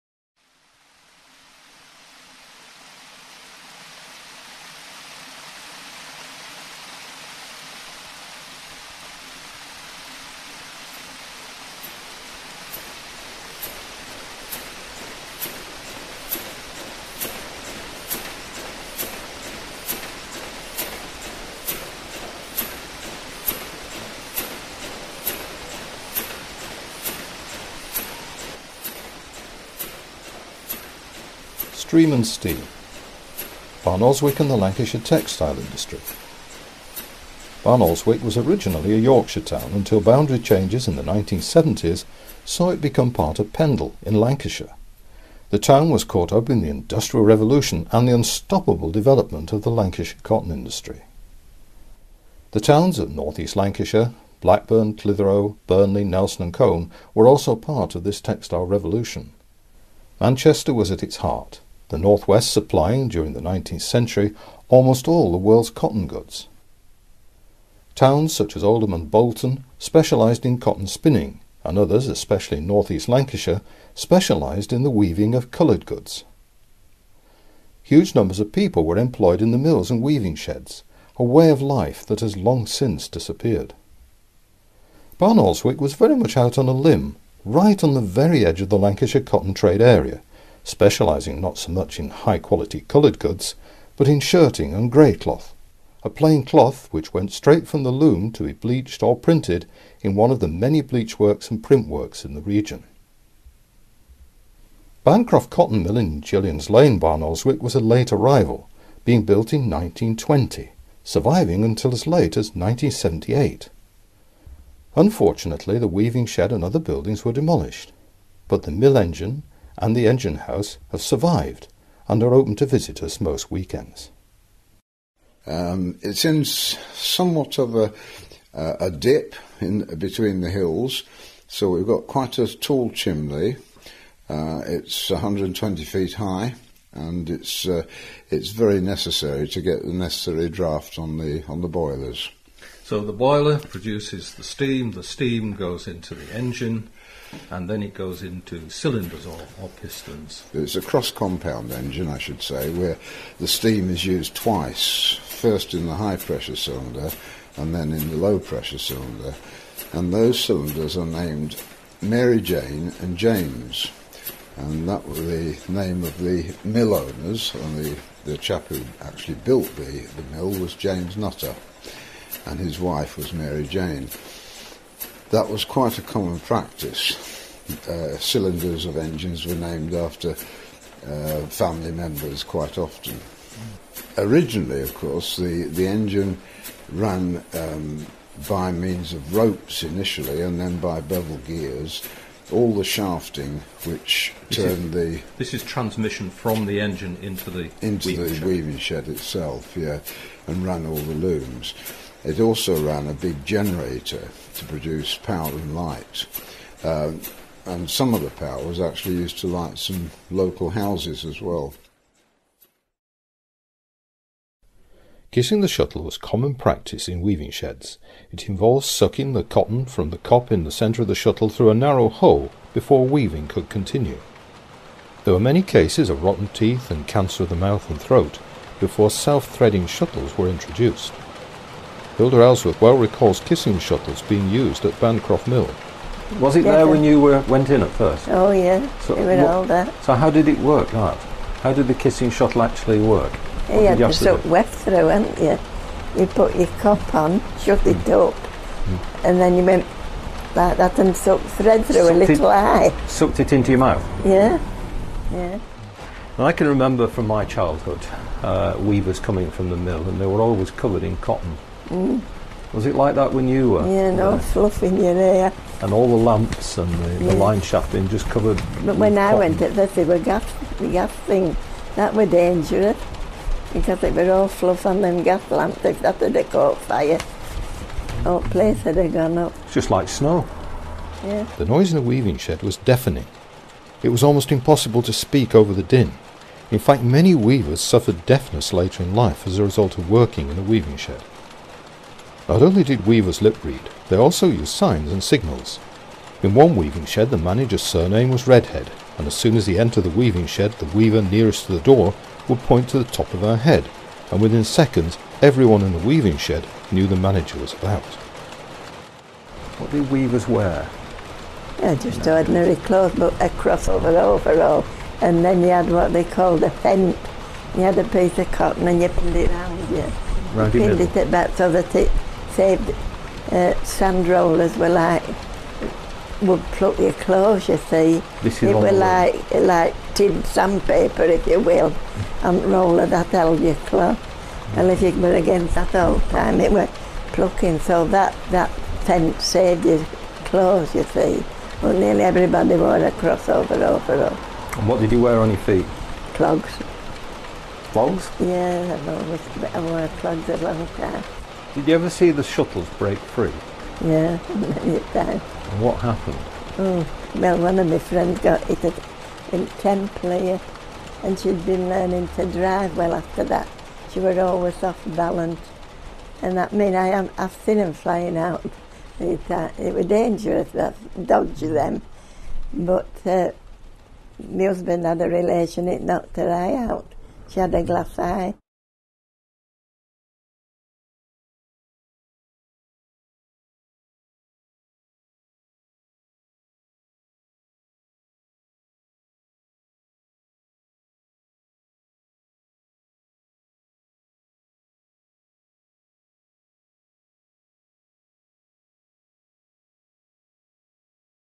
Barnoldswick and the Lancashire Textile Industry PART 1
The 1st of two short presentations.  The mill engine is explained and a former cotton weaver in the mill recalls what it was like to work there.